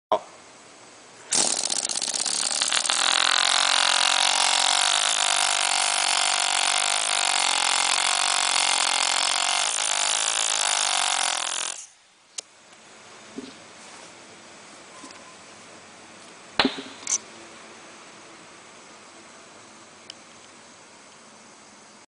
AL4429 Air Operated Valve Grinding Lapping Tool
This air valve lapper improves efficiency in lapping of valves by the use of air pressure of a compressor.
Since required up-and-down motion is given by adjusting air pressure, this lapper can be used extensively for a smaller valve to a larger valve.
With an adjustable speed regulator for precise control during operation.
Come with rubber suction cups : 20, 30, 35, 45mm
Designed for operating in tough environments.
Type: Front Exhaust
Variable Speed: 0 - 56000 rpm
Air Consumption: 3.53 CFM (100 L/MIN)
Working Pressure: 90 PSI (6.3 BAR)
Air inlet: 1/4"
Weight: 1.5 KG (3.3 LBS)